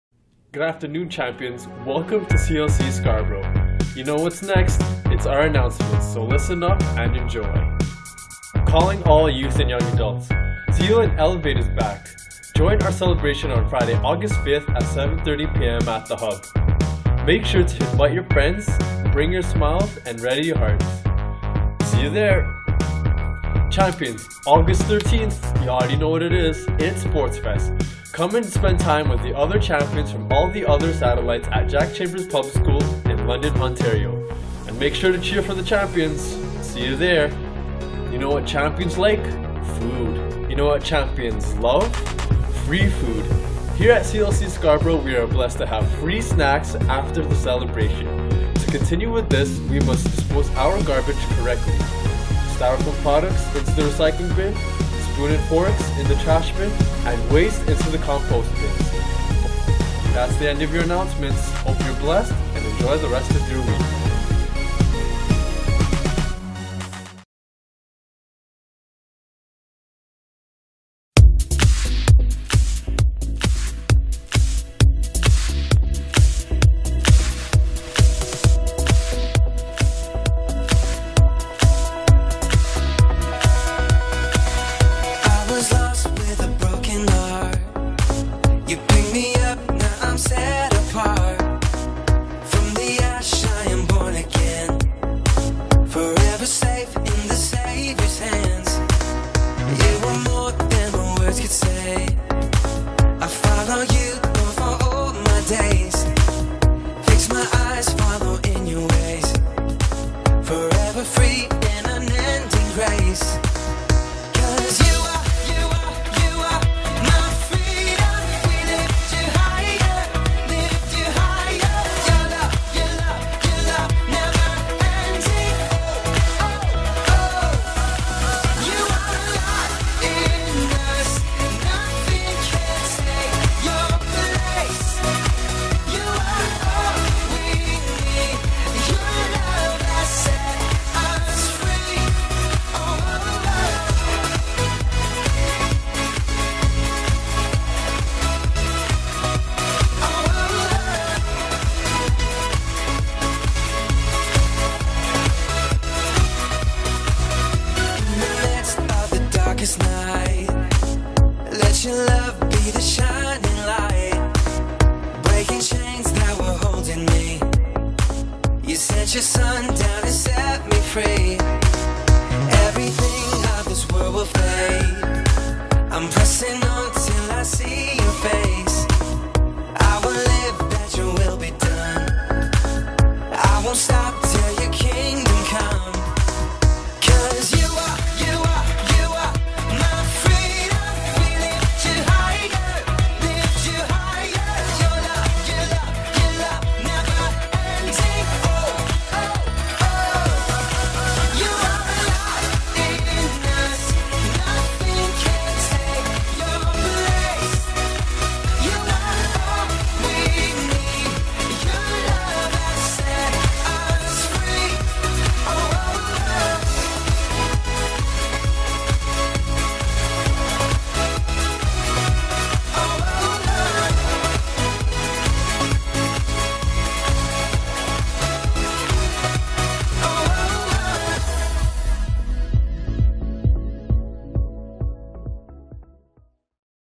CLC-S - July 24, 2016 Announcements & Camp 2016 Video
Champion Life Centre - Scarborough
"EMPOWERING LIVES TO IMPACT OUR WORLD"
Sunday Celebration - July 24, 2016 3:00 PM
Warden Full Gospel Assembly
2210 Warden Avenue

1. Announcements
2. Camp 2016 Video